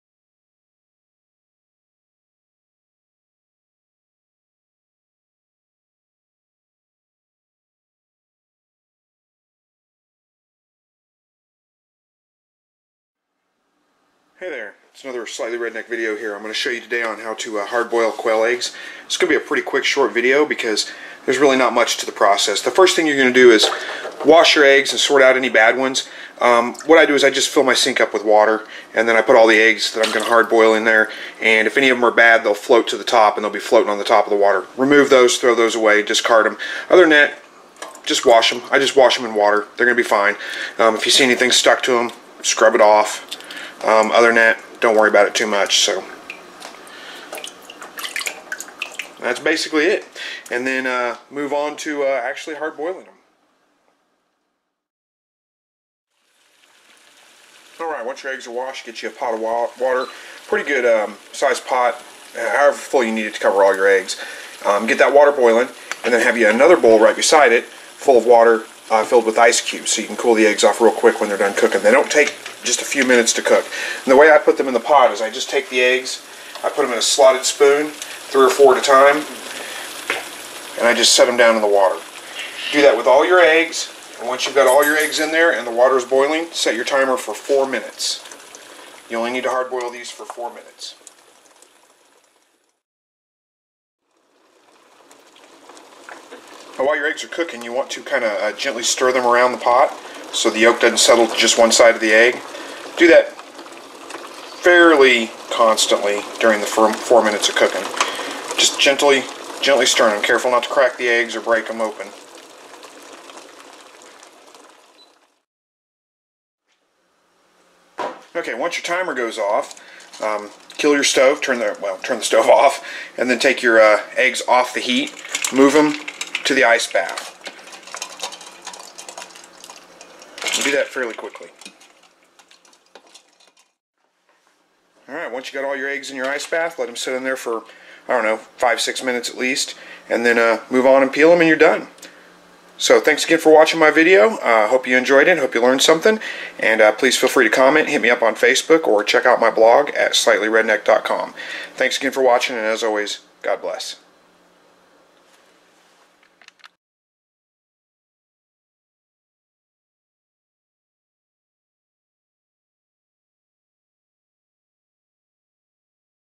How to Boil Quail Eggs
I made some deviled quail eggs for my sisters birthday so I thought I would share a short video on hard boiling quail eggs.  I am sorry about the poor lighting.  My kitchen is not the best filming location.

They are pretty easy to hard boil but you do have to pay attention.  They are done in 4 minutes and need to be stirred frequently.

Feel free to hit me up on facebook and share your tips.